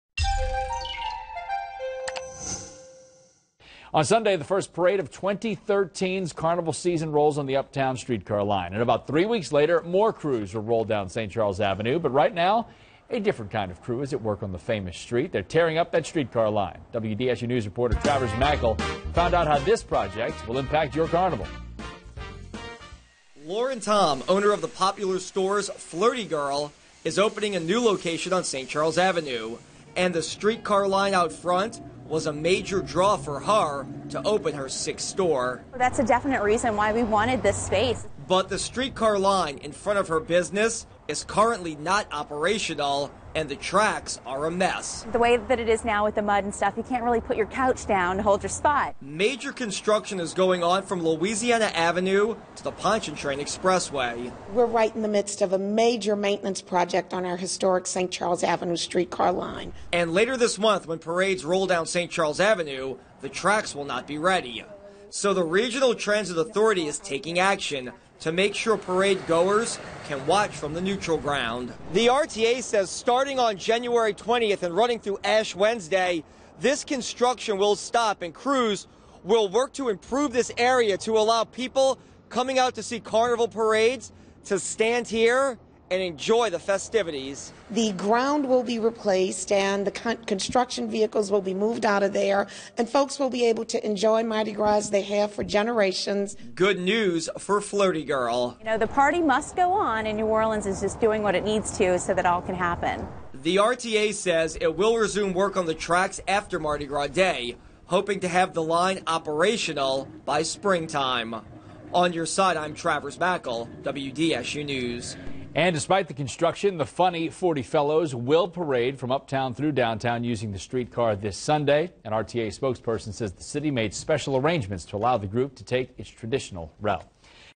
RTA works to clear neutral ground on St. Charles Avenue by Carnival
Construction on a major maintenance project is going on from Louisiana Avenue to the Pontchartrain Expressway.